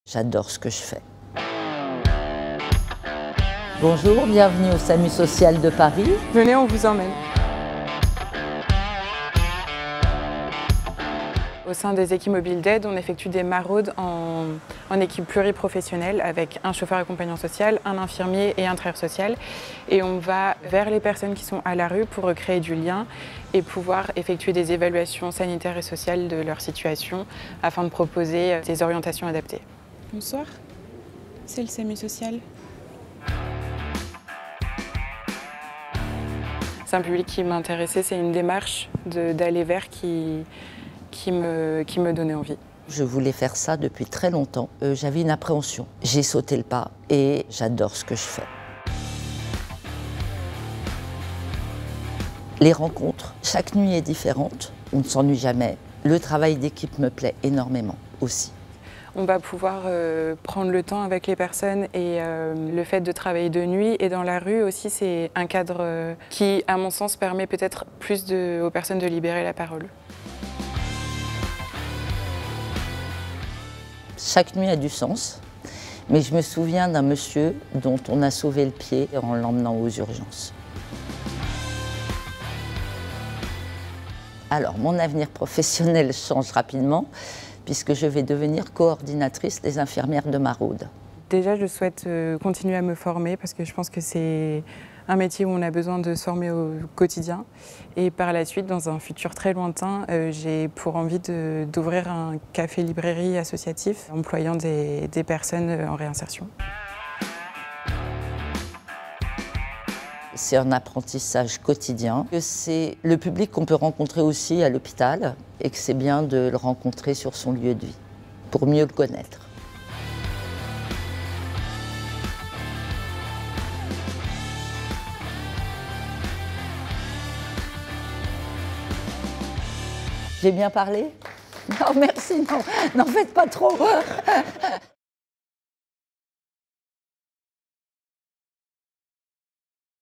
Infirmier(ère) et travailleur social en équipe mobile : pourquoi en maraude de nuit ?
AuServiceDesAutres – Laurine et Nathalie nous présentent leurs métiers en équipe mobile de nuit, au SAMU social de Paris.

Mais c’est quoi une équipe mobile ? Une équipe mobile est composée à minima d’un infirmier et d’un travailleur social. Cette équipe intervient sur initiative propre, sur sollicitation extérieure ou sur demande de l’ARS. Elle assure les premiers soins (bilans de santé, accompagnement à l’ouverture de droits…), des soins infirmiers à domicile, de l’éducation à la santé, ainsi qu’un suivi des patients chroniques pour coordonner leurs parcours en santé et les accompagner dans les démarches de la vie quotidienne.

Pour en savoir plus sur les métiers et les structures du médico-social